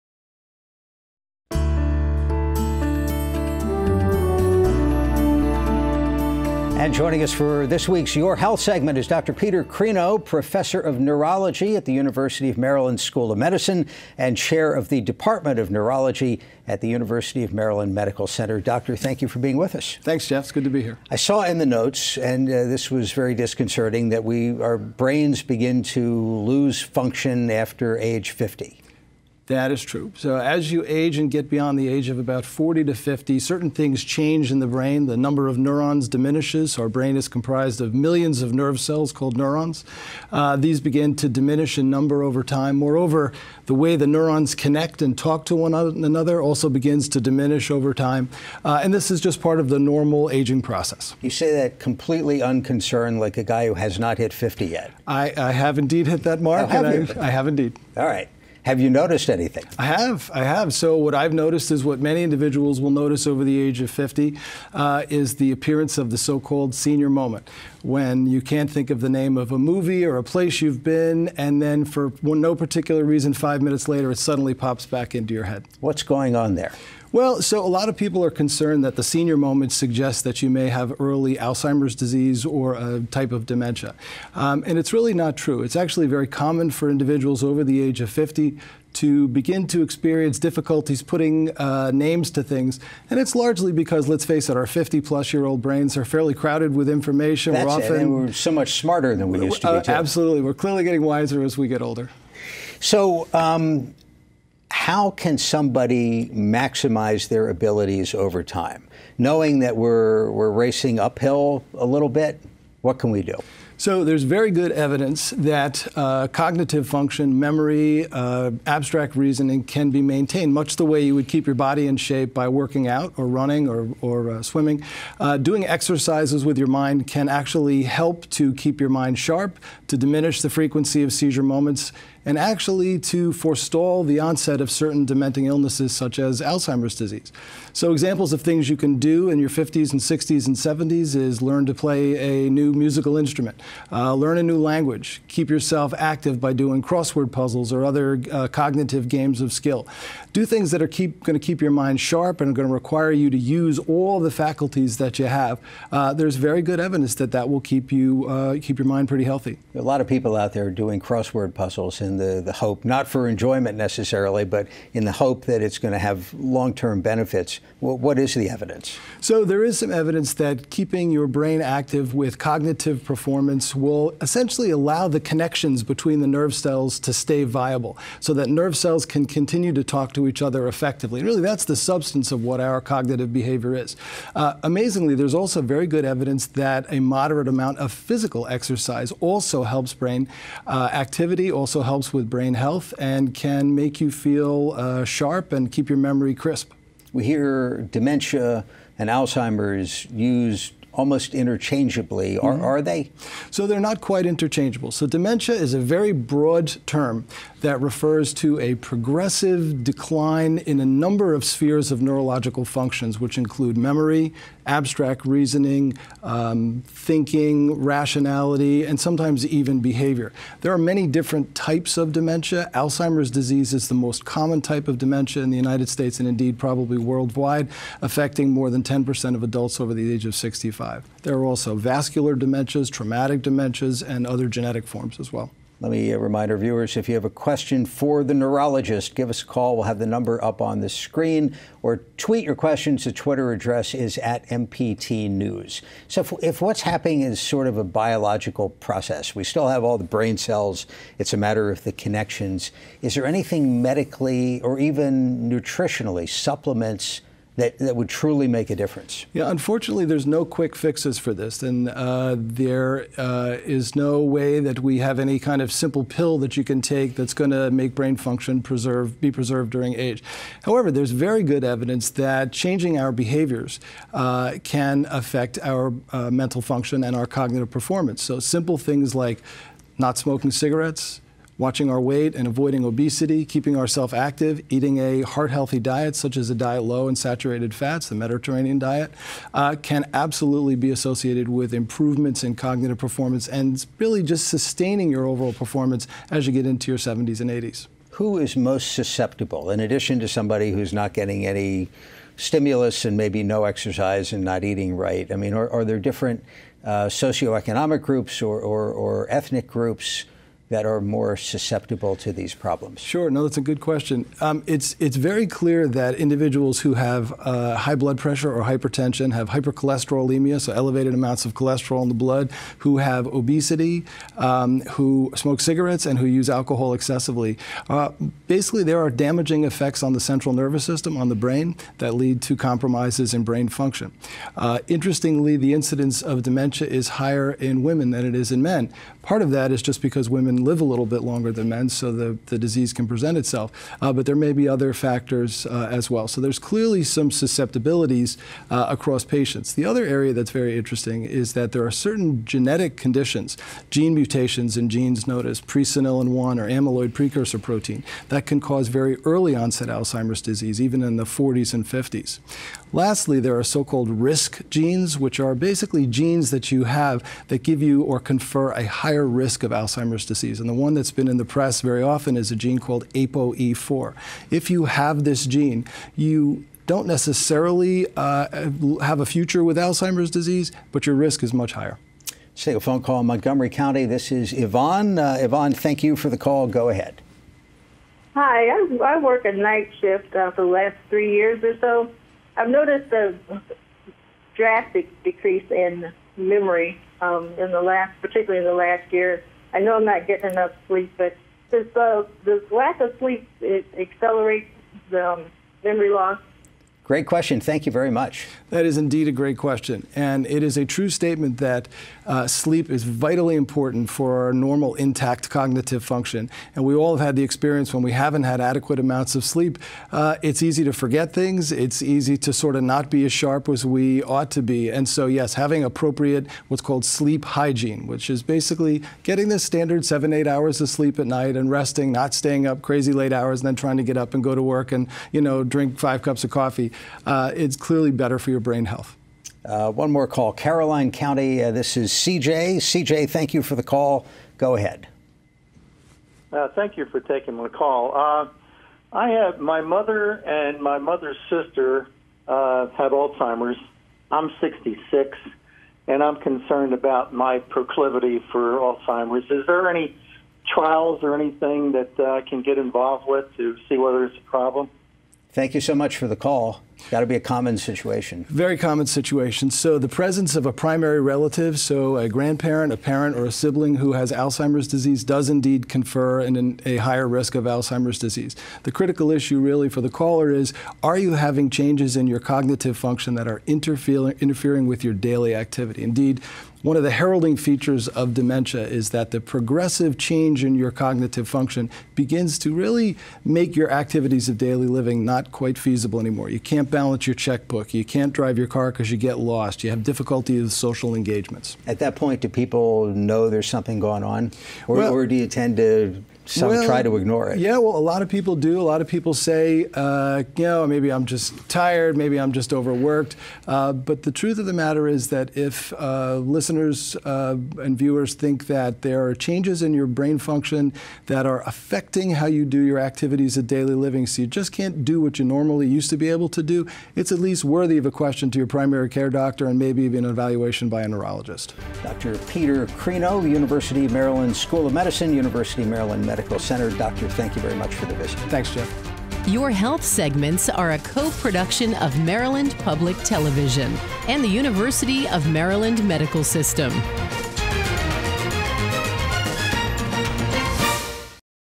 Your Health: Intelligent Aging
In this Your Health segment, Peter Crino, MD, PhD, an internationally renowned neuroscientist and chair of neurology at the University of Maryland Medical Center, discusses how our brains change as we get older, ways to keep your brain healthy as you age, and the differences between having a "senior moment" and a serious neurobiological disorder.